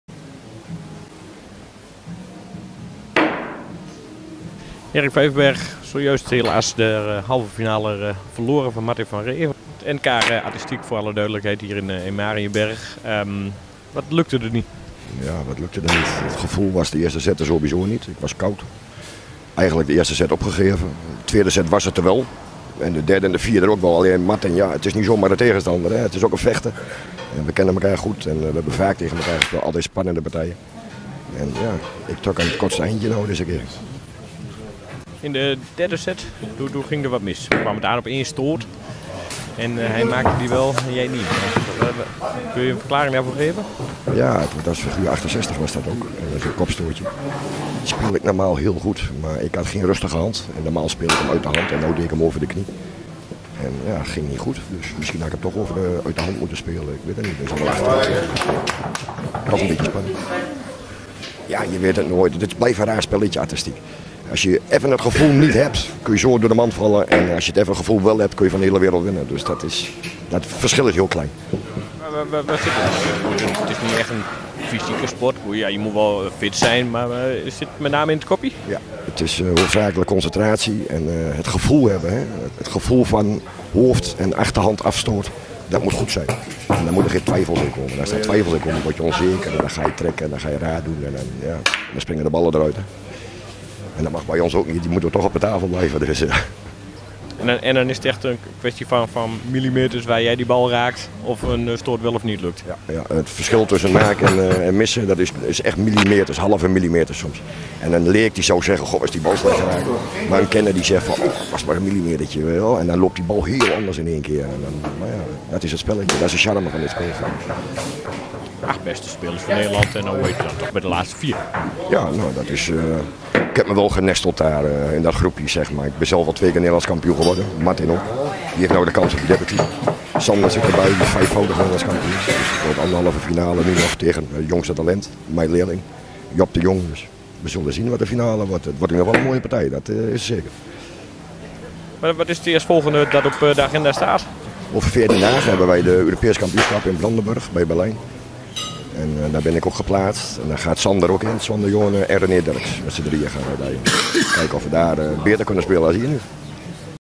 Biljarter Erik Vijverberg strandt in halve finale op NK Artistiek
Zondag 19 april vond in Mariënberg het Nederlands kampioenschap biljart artistiek plaats. Deze vorm van biljart wordt ook wel kunststoten genoemd.

Sportman het jaar 2014 Erik Vijverberg stond in de halve finale tegen Martin van Rhee.